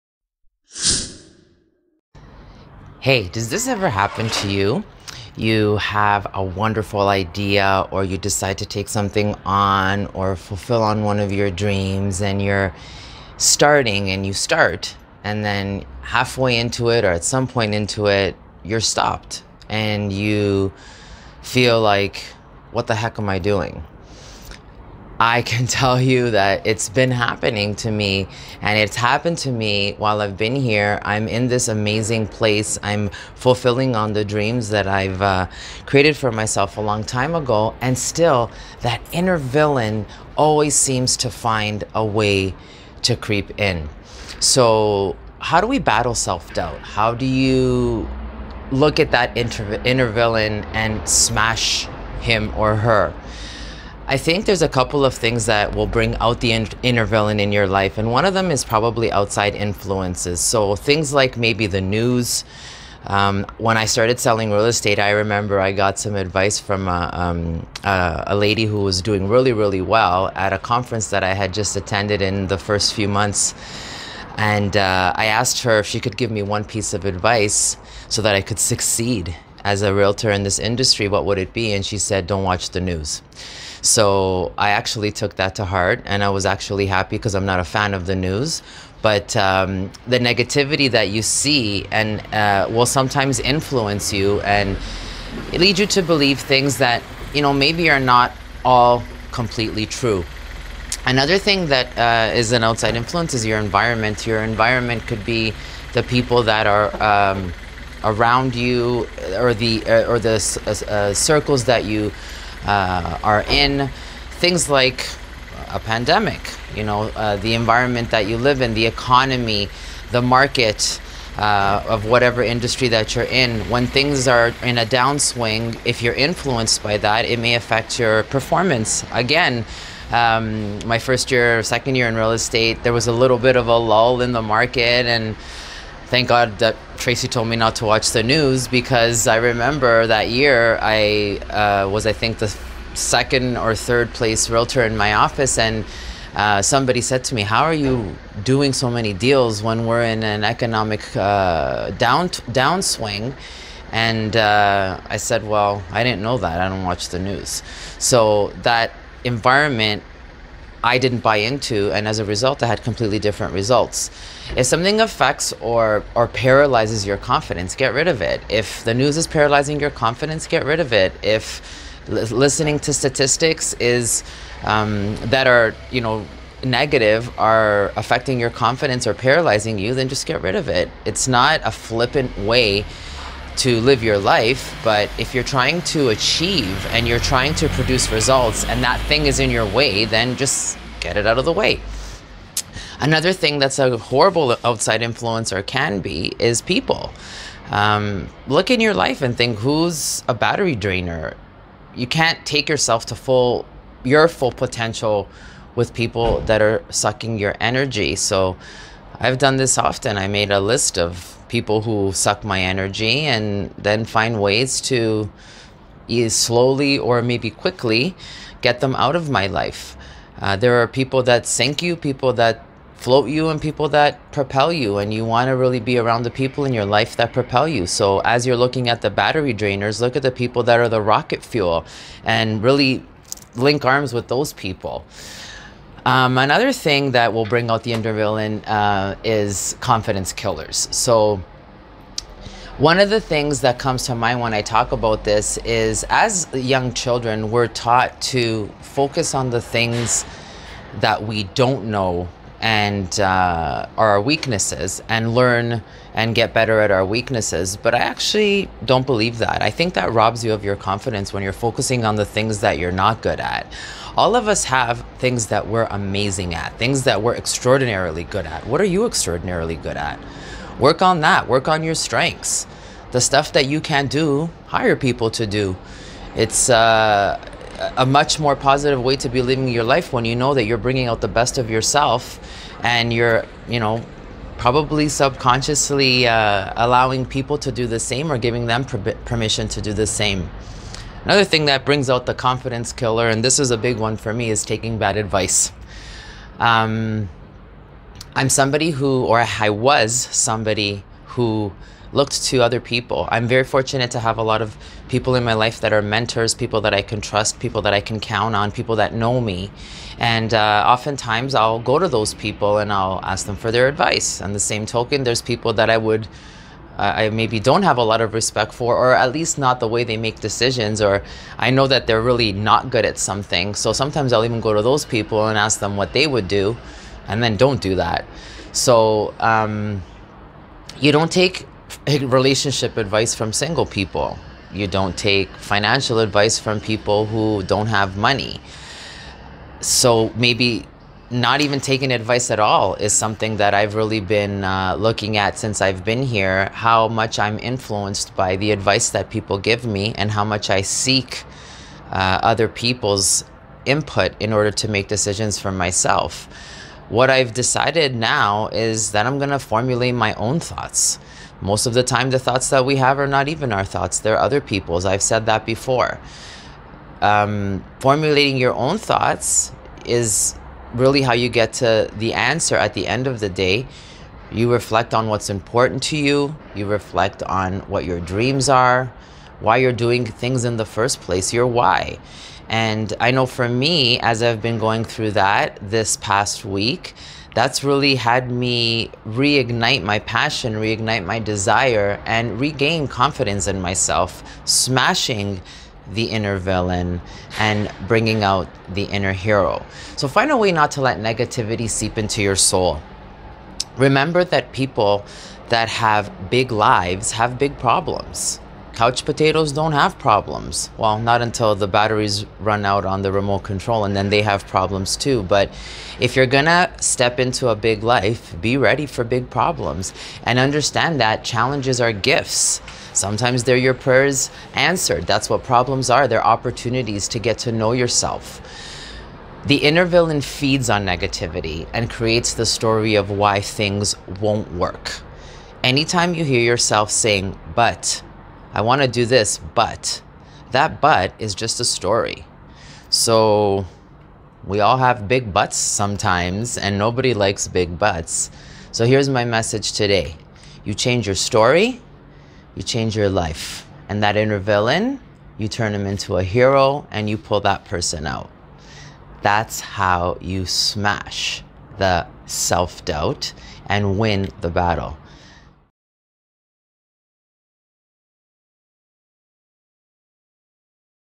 FUTURE by DESIGN - Ep. 013 "BATTLING SELF-DOUBT"
Ever notice that once you create big visions for yourself and begin to execute them, how the "Inner Villain" starts to creep in to tell you that you are NEVER GOING TO BE ABLE TO DO THAT?
Self-doubt is a challenge for many great minds (whether they admit it or not) and if you aren't aware that this is just a step along the roadway to success and fulfilment, it can easily smash any big dream or goal.
How do you recognise that "Inner Villain" and what can you do to take the power back?
Could FUTURE by DESIGN be an answer to help you find the answers within???